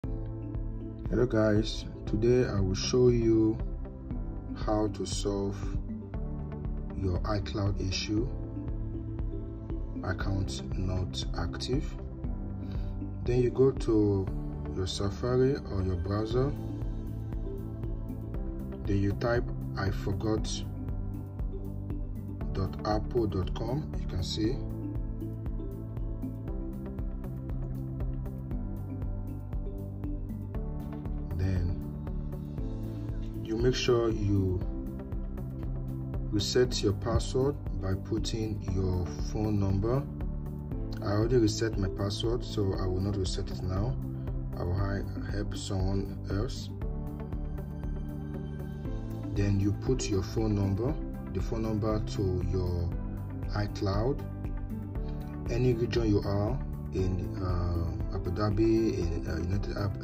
How to Fix Apple Account Not Active Issue in 1 minute (2024)
Are you facing the frustrating **"Apple Account Not Active" issue** on your iPhone, iPad, or Mac? Don’t worry—this quick tutorial will show you how to fix it in just **1 minute**! In this step-by-step guide, I’ll walk you through the exact process to solve this issue in 2024. Whether you’re a **new YouTuber**, a **small YouTuber**, or simply someone trying to fix their **iCloud account**, this solution works like a charm.

In this video, you’ll learn:

- Why your **Apple ID says ‘Not Active’**
- The most common reasons behind the **Apple Account Not Active error**
- A fast and effective fix for **iCloud login issues**
- How to regain access to your Apple services like iMessage, App Store, and iCloud

If you’ve been searching for answers, this video is perfect for anyone troubleshooting **Apple ID issues**, **Apple account recovery**, or **iCloud sign-in errors**. No tech expertise required—this solution is beginner-friendly and works across all Apple devices.

Go to this new video for better updates